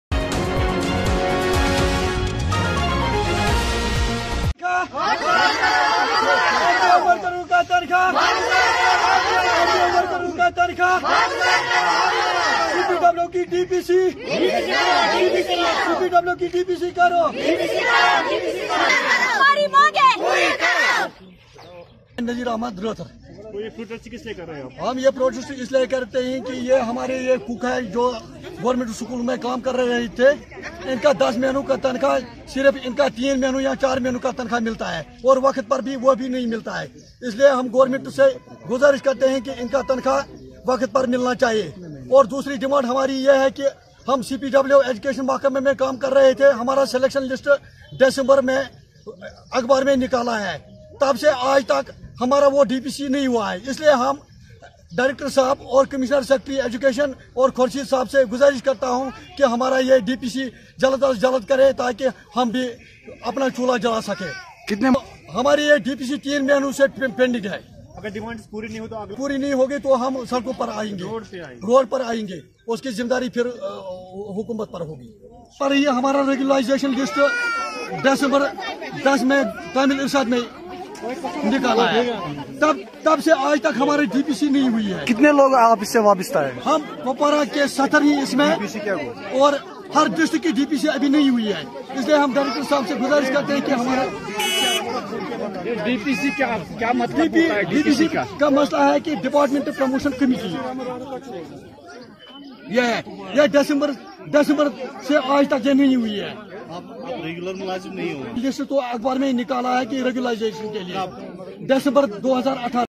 REPORTERS PAGE- हमें नियमित किया जाए, प्रदर्शनकारी, वेतन और नियमितीकरण की मांग
लंबित वेतन और नियमितीकरण की मांग को लेकर कई सरकारी स्कूलों के रसोइयों ने प्रेस एनक्लेव श्रीनगर में विरोध प्रदर्शन किया